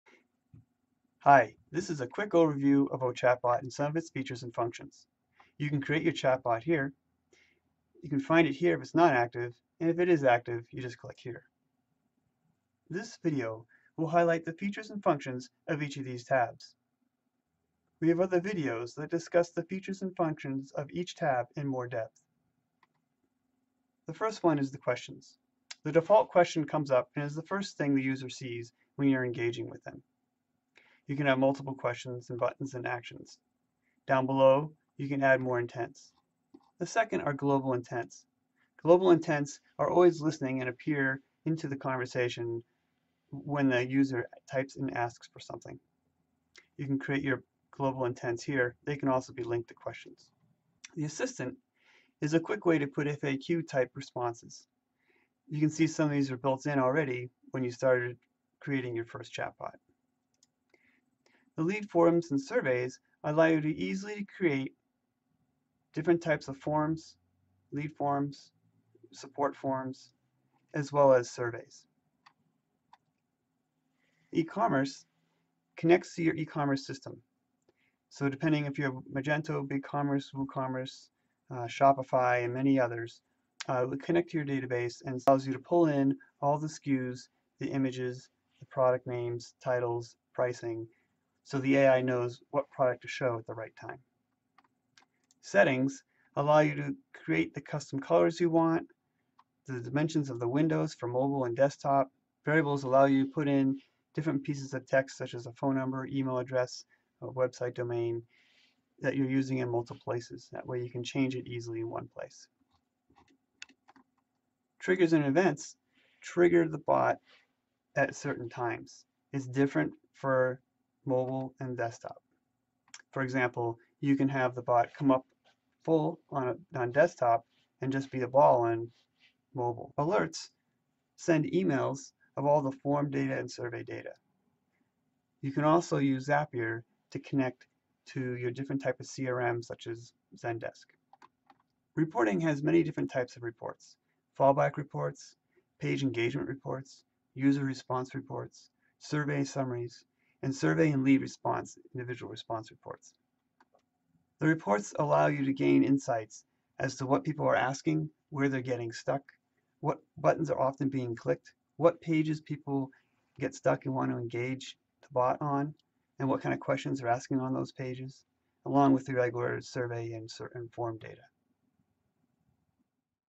Ometrics Ochatbot Quick Overview
This is a quick overview of Ochatbot features and functions.